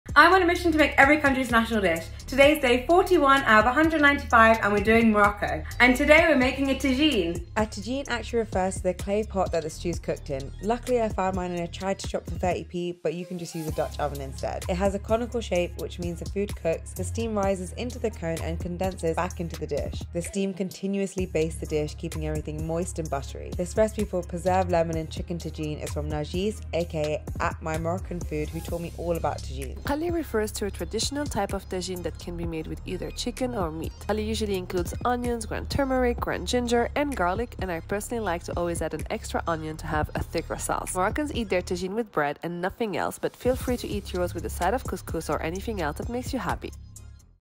Tagine - National Dish of Morocco (Day 41/ 195)
Honestly, this was so much more delicious than I was ever expecting. It just shows how you don't need a million different ingredients to make something incredible.